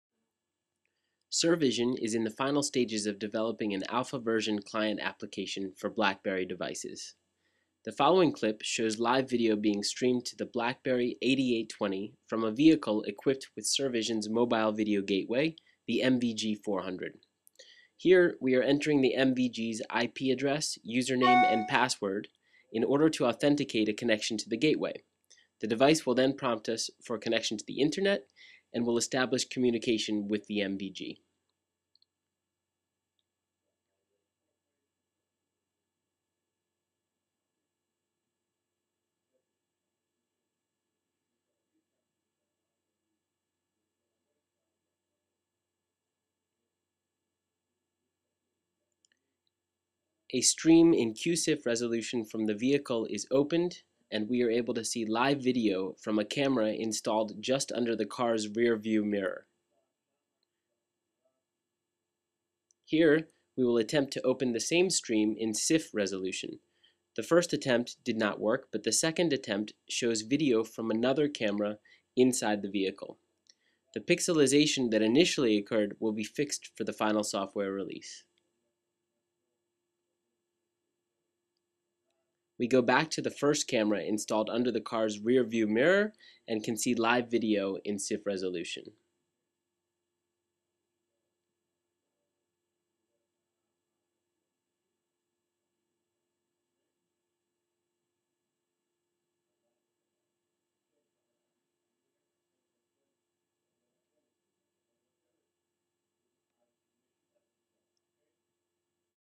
SerVision Client Application for BlackBerry
SerVision's Alpha ver. Client Application for BlackBerry devices which enables the viewing of live video streams from remote sites and vehicles on handheld BlackBerry devices.